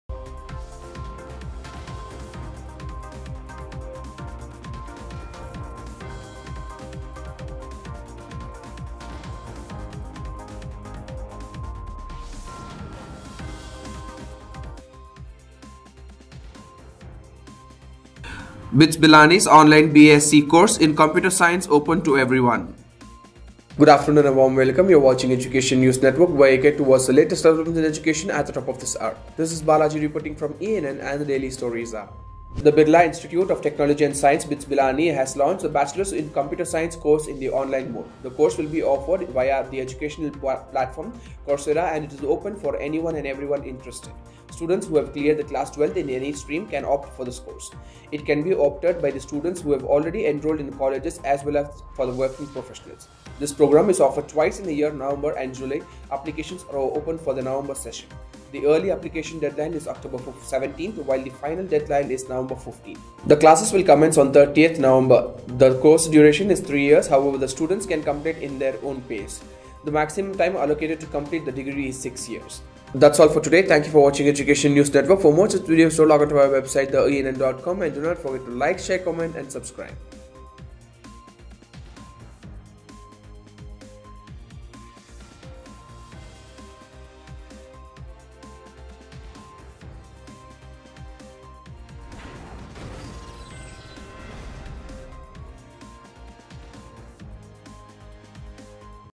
BITS Pilani’s Online BSc Course in Computer Science open to everyone
The Birla Institute of Technology & Science (BITS) Pilani has launched a Bachelor’s in Computer Science course in the online mode. The course will be offered via the educational platform Coursera and is open to anyone and everyone interested. Students who have cleared their class 12 in any stream can opt for the course. It can be opted for by students who have already enrolled in a college as well as working professionals.
This programme is offered twice a year in November and July. Applications are open for the November session. The early application deadline is October 17 while the final deadline is November 15. The classes will commence on November 30. The course duration is three years; however, students can complete it at their own pace. The maximum time allocated to complete the degree is six years.